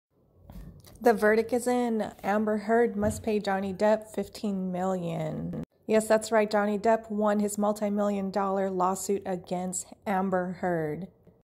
Verdict in| Johnny Depp awarded $15M!
Correction, he won $10.350M.  Punitive cap in Virginia is $350K (Judy originally awarded $5M)